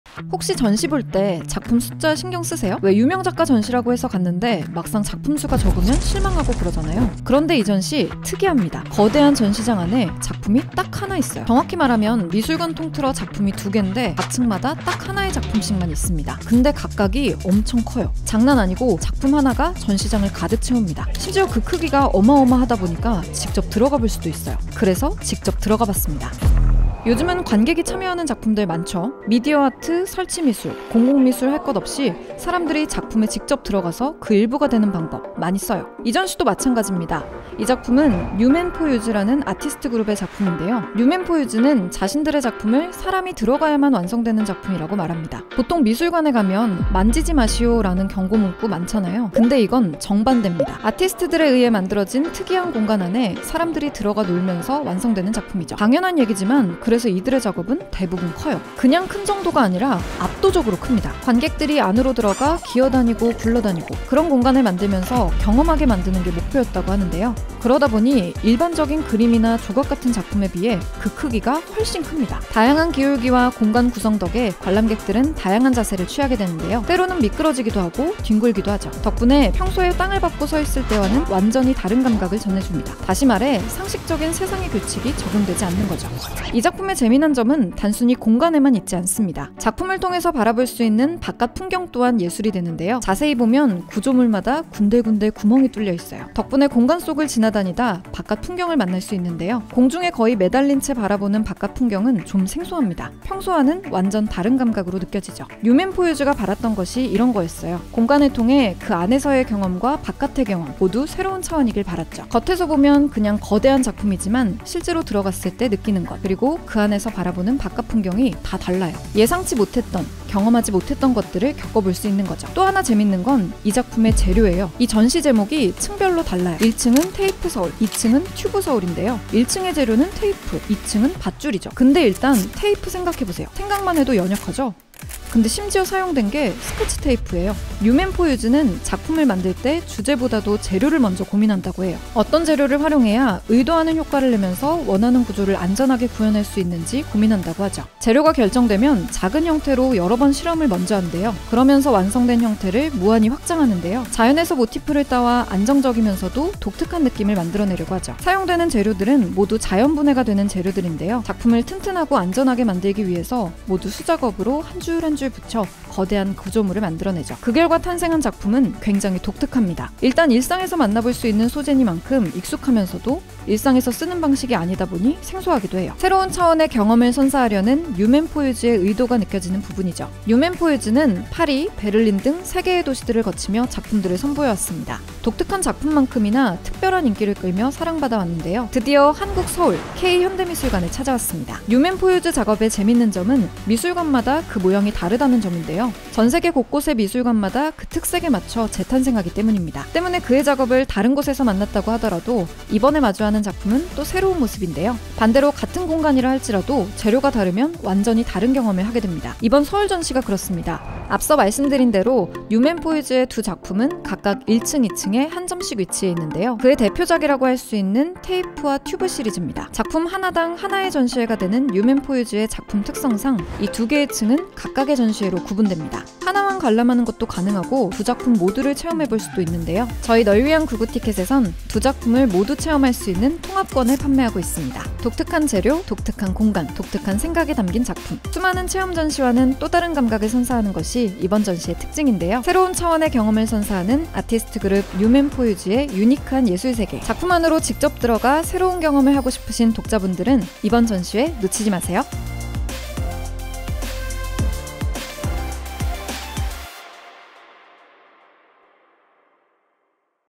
드디어 한국에 상륙한 압도적 스케일 전시 ㄷㄷ
널위문 구독자 분들을 위해
[테이프 서울] 최.저.가 티켓을 판매합니다.
이번 티켓은 K현대미술관의 1.2층을
전부 관람할 수 있는 통합권입니다.
선착순으로 판매되니 놓치지 마세요!
거대한 전시 공간에 작품이 딱 하나?
압도적인 크기의 작품에 직접 들어가는
이색 전시회에 초대합니다.